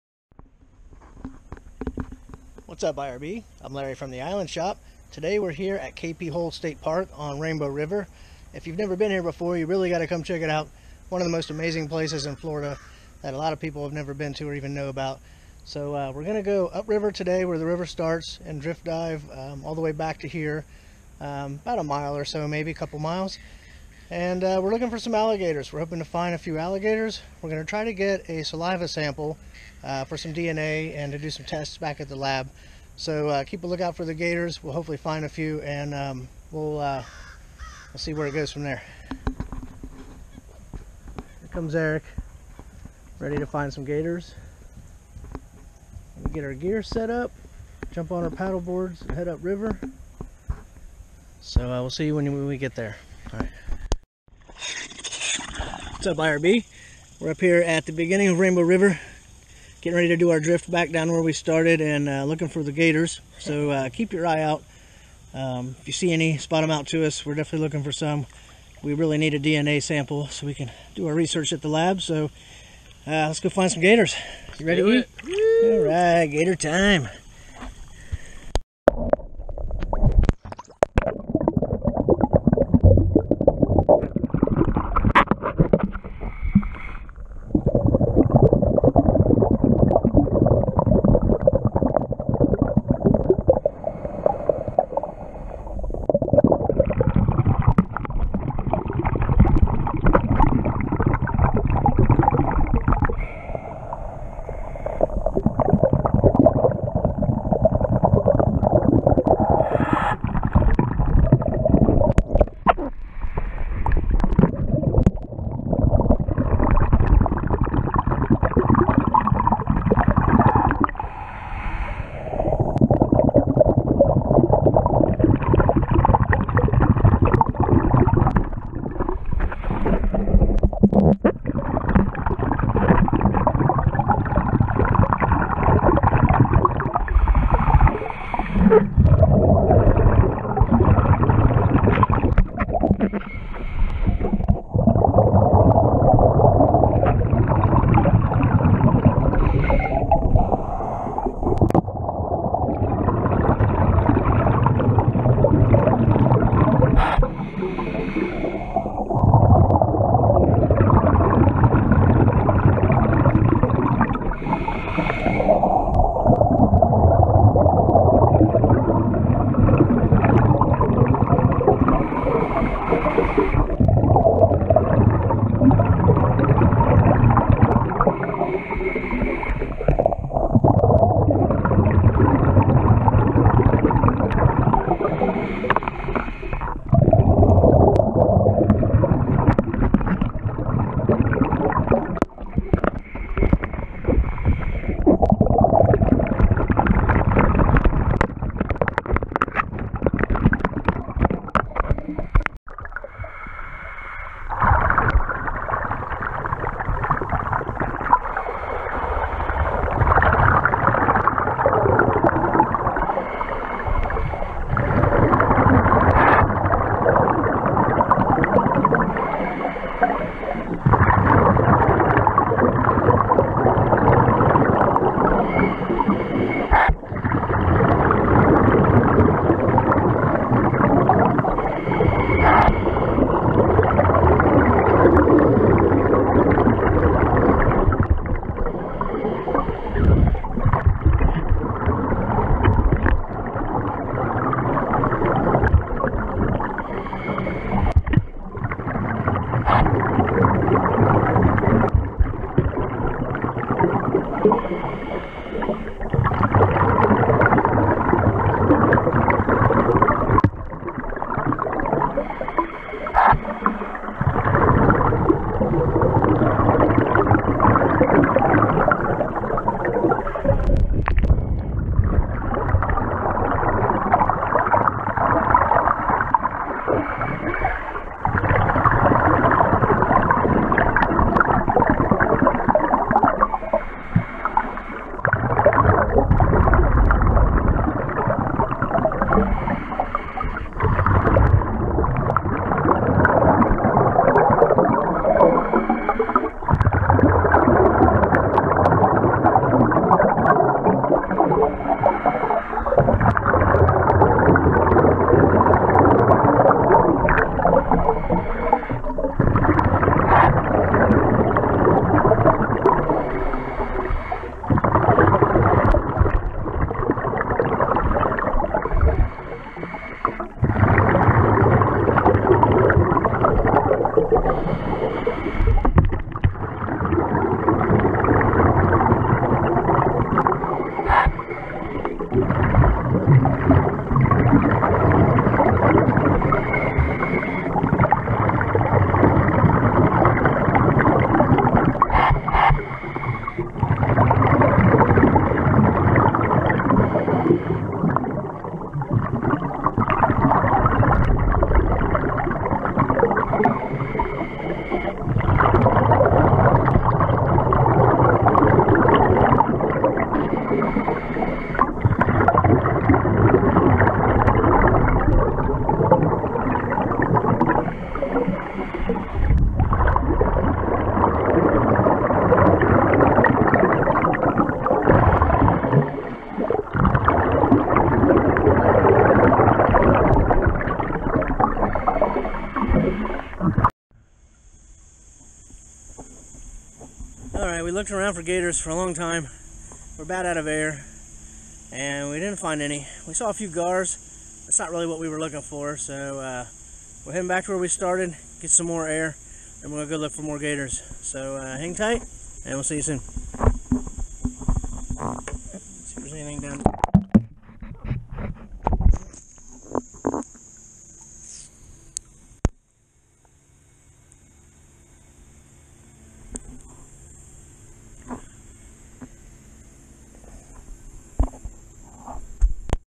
Alligator Research , Watch out for gators!
Trying to get a cheek swab from an alligator so we can study the DNA